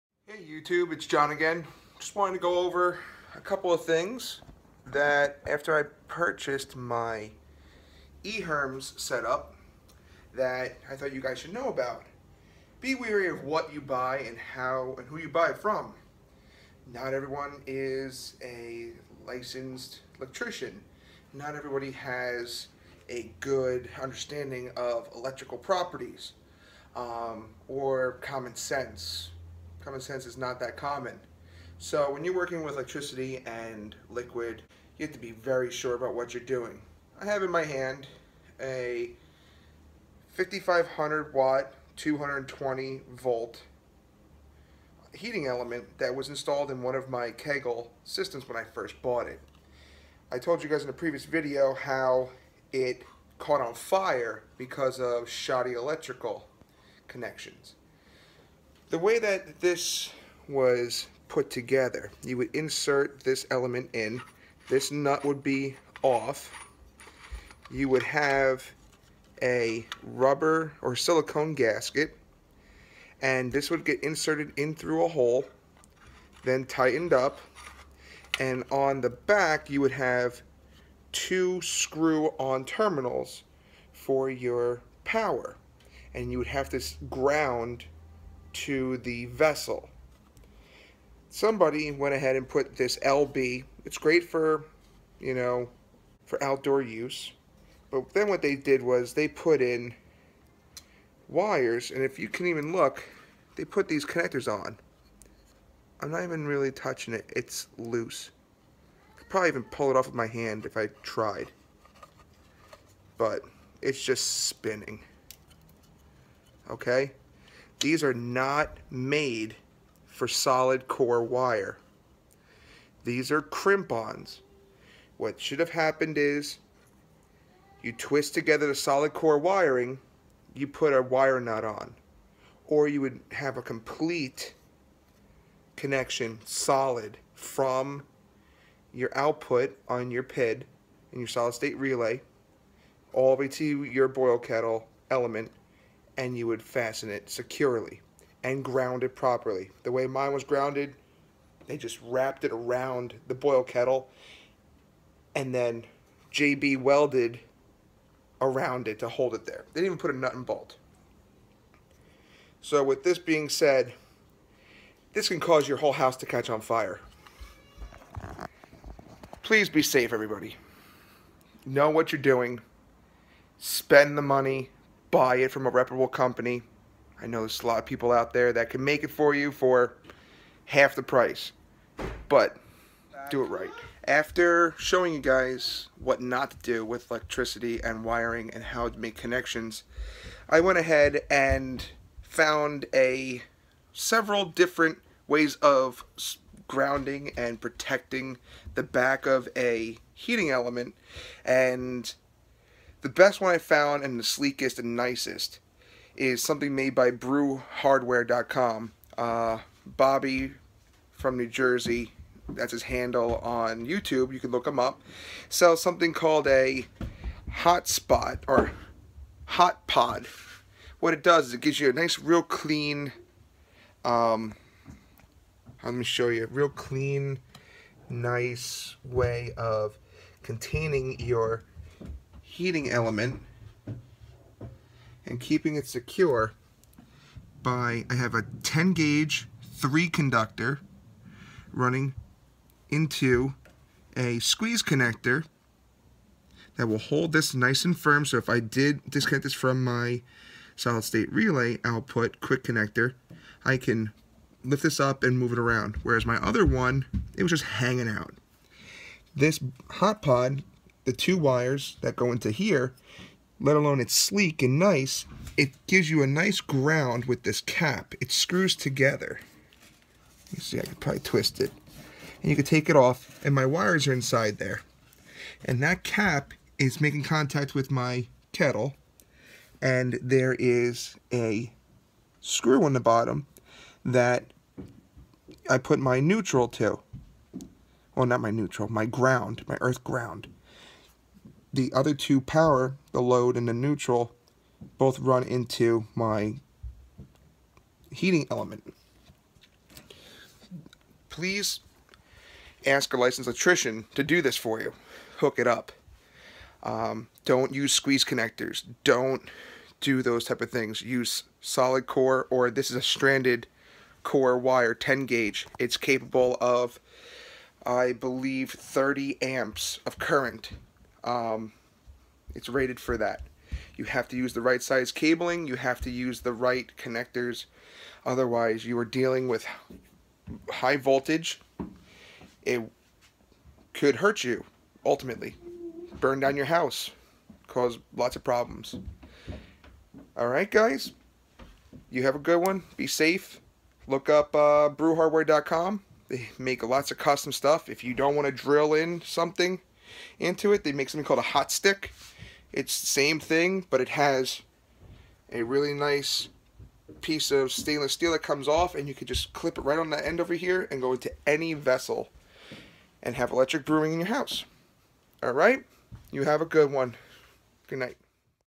How not to wire a heating element
After picking up a used eHERMS system I ran into a bunch of issues that caused me to spend more than I wanted. Please be safe everyone.